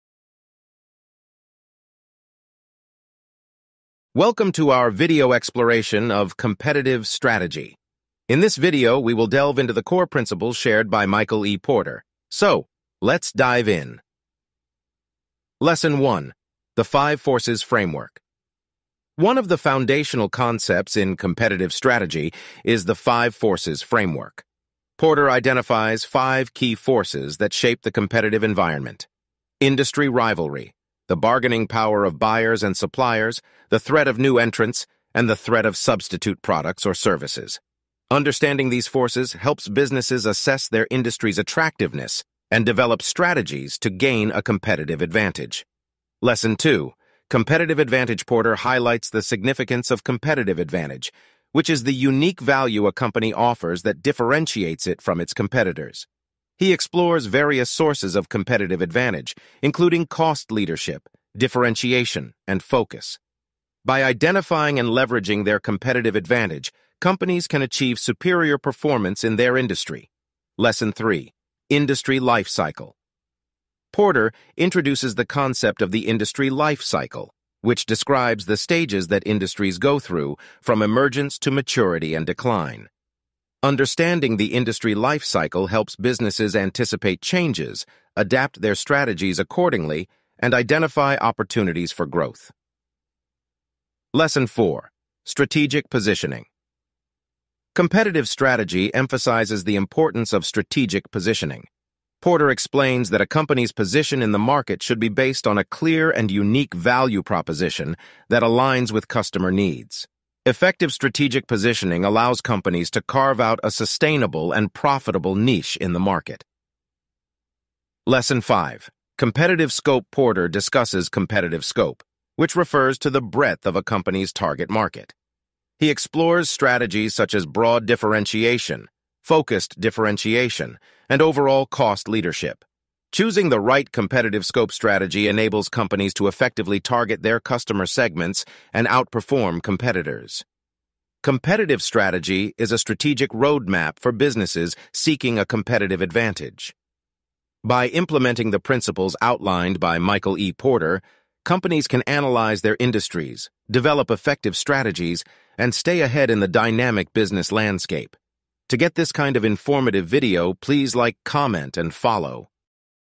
Review on "Competitive Strategy"
The business industry is so huge that we constantly compete with others. So, you can learn how to deal with your competitors and make a huge difference in the industry.